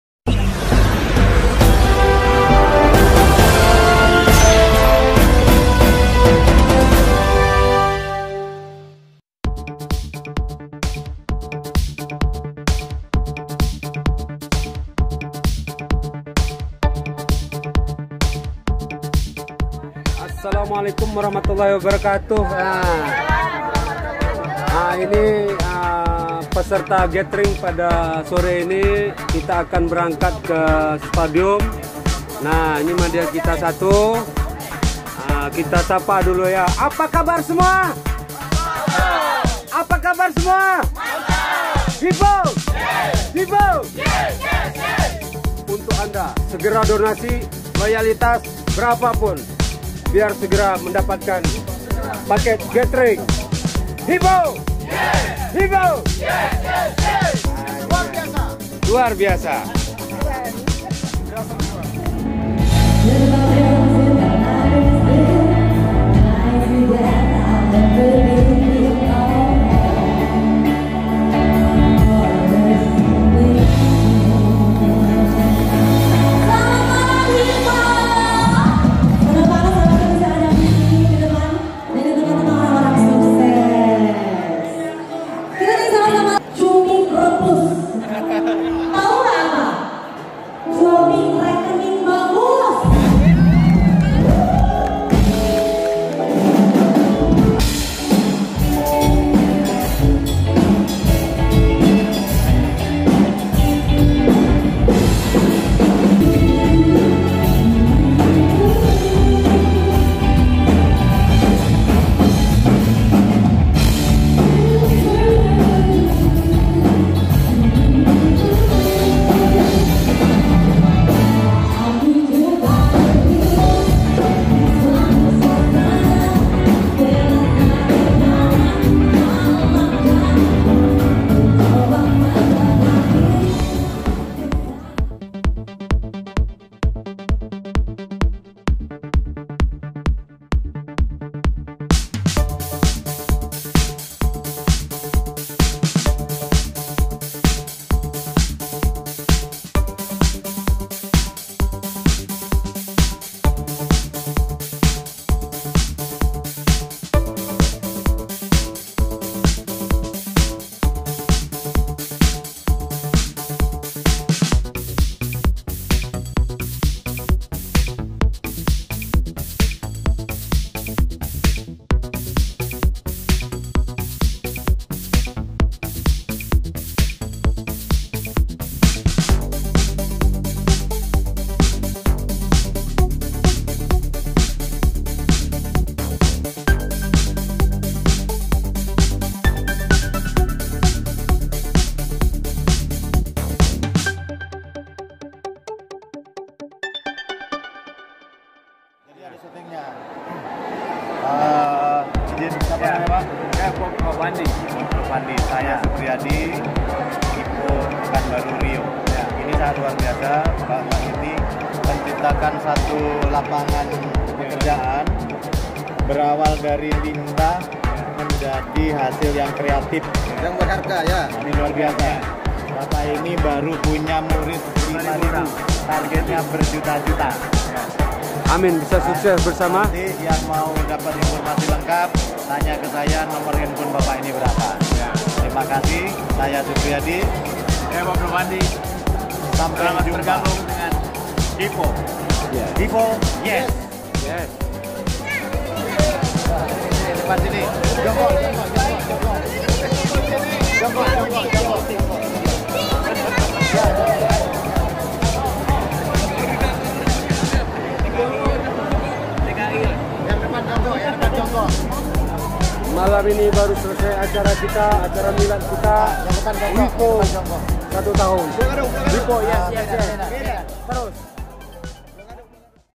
ANTUSIAS MILAD PERTAMA HIPO JAKARTA
Baru pertama ulang tahun hipo sudah luarbiasa dahsat bagai mana selanjutnya